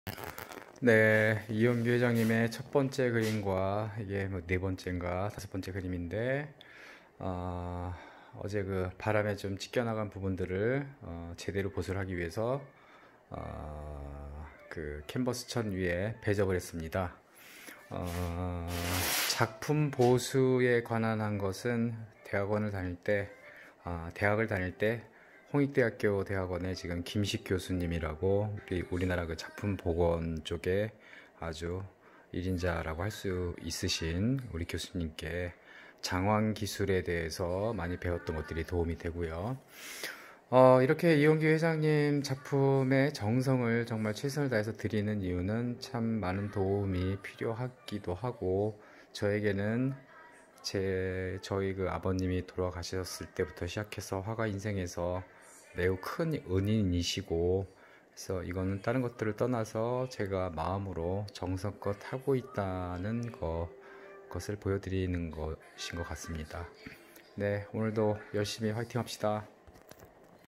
■(2020.05.29)작가 정수일-이홍기 회장님 초상화-작품 보수를 위한 배접.
바람에 찢겨나간 부분을 보수하기위해서 캔버스 아사천에 배접을 했다. 장지가 아닌 아사천을 택한 이유는 원 화면을 그린 아로쉬지의 엄청난 장력을 버티기 위함이다.  이홍기 회장님은 나의 화가 인생 최고의 위기때 구세주처럼 나타나셔서 손을 내밀어주신 은인이시다. 이번  이홍기 회장님의 작품들에 공을 들이는 이유는 그 고마움들을 마음에 담고 싶어서이다.